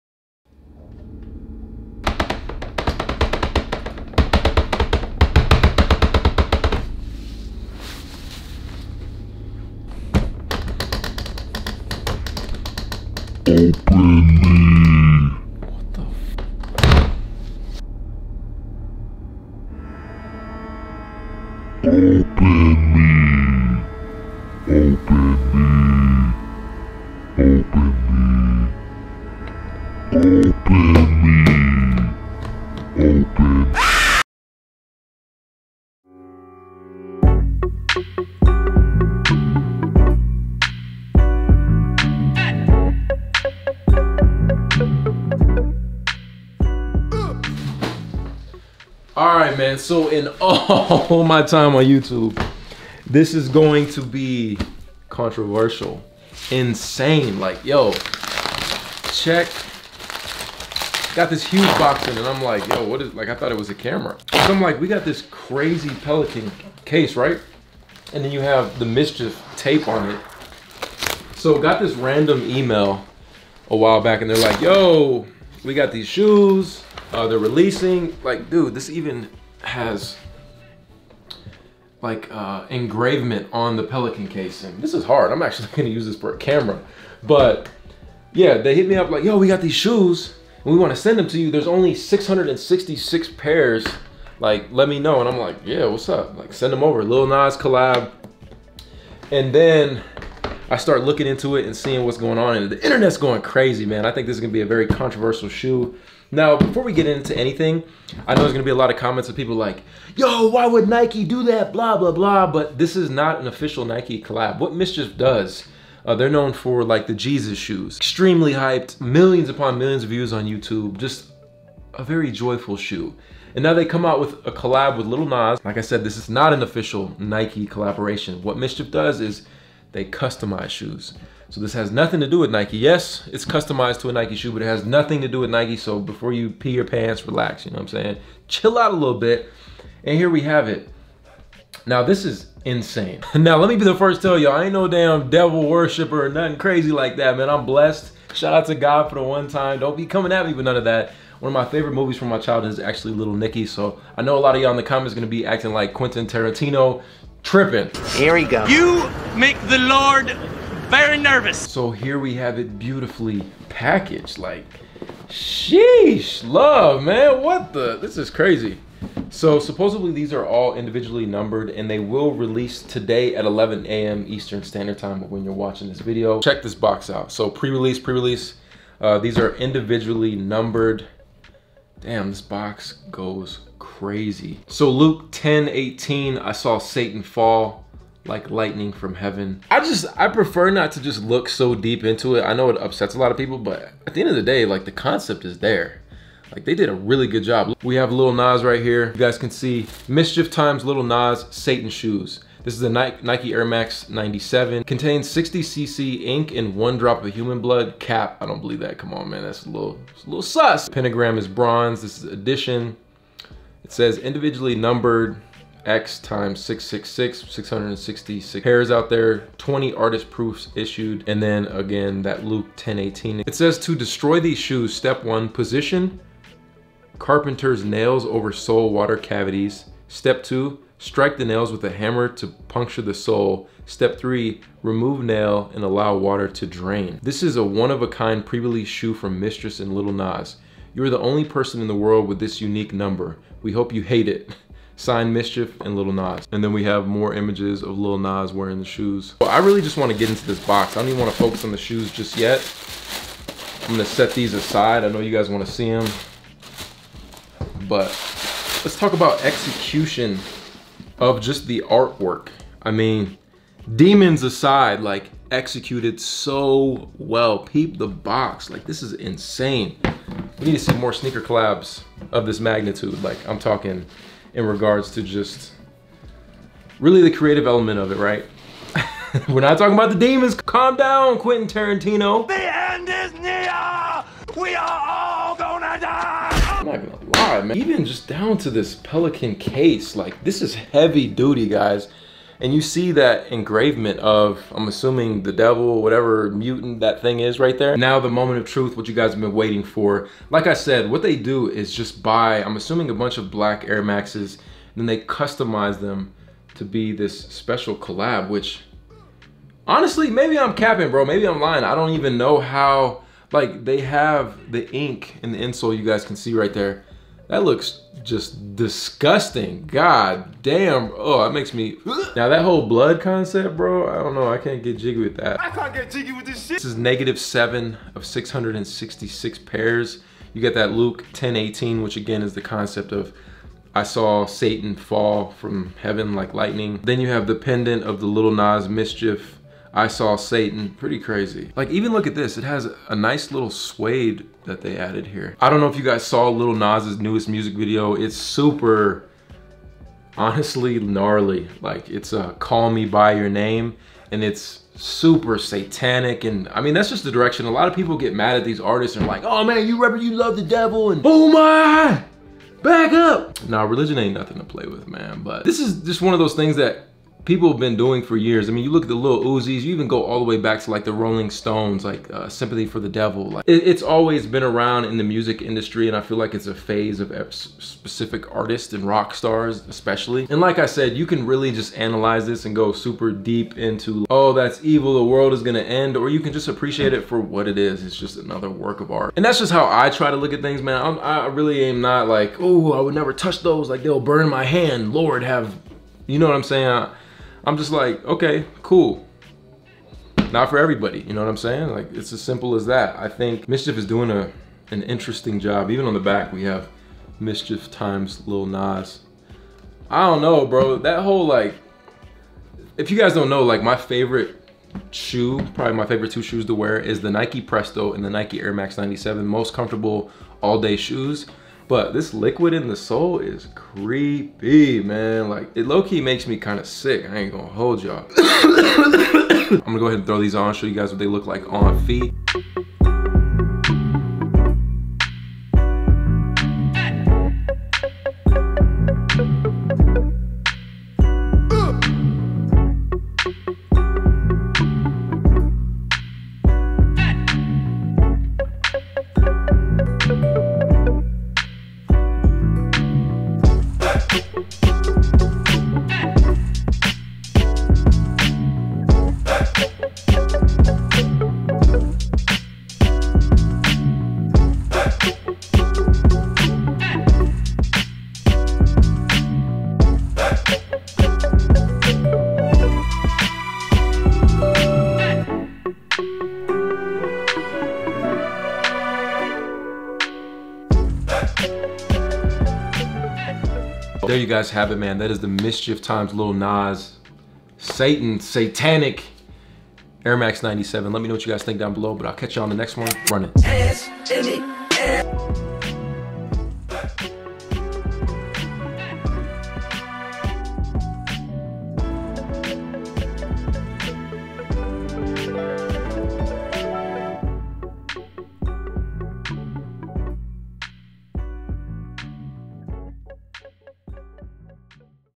$1,018 Nike "Satan Shoes" by Lil Nas X Unboxing
The very unique and limited Satan Shoe is a base Nike Air Max 97 by MSCHF and Lil Nas X, with the air bubble containing 60cc ink and one drop of human blood. Each shoe is uniquely numbered between 1/666 and 666/666 and sells for $1,018. In addition, each shoe has the bible verse Luke 10:18, an upside-down cross, and a handmade bronze pentagram with custom engraving. Satan Shoes Instagram